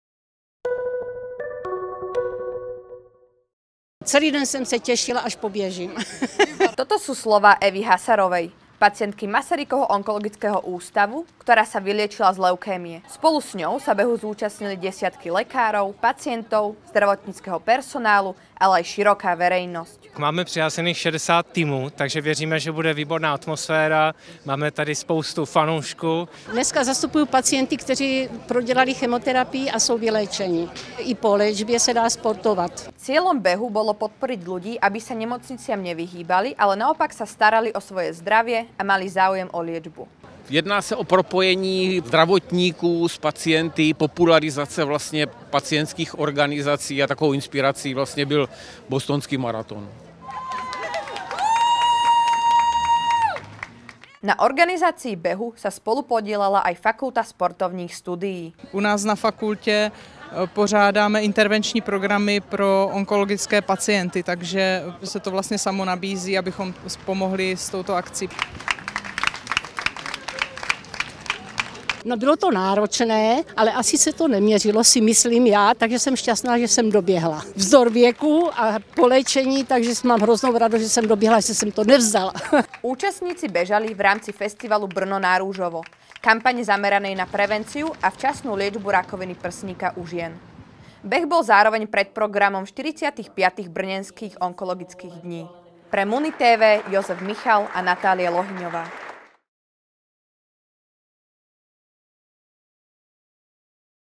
🎽 Bostonský maraton v brněnském stylu spojoval pacienty a lékaře
Masarykův onkologický ústav a Fakulta sportovních studií MU usporiadali historicky prvý ročník behu na podporu pacientov s rakovinou. Bežalo sa v parku pod Janáčkovým divadlom formou štafety dvojčlenných tímov. Každý z bežcov odbehol približne 2km. Cieľom tohto podujatia bolo odbúranie bariér medzi lekármi a pacientmi.

Natáčeli: Josef Michal a Natália Lohyňová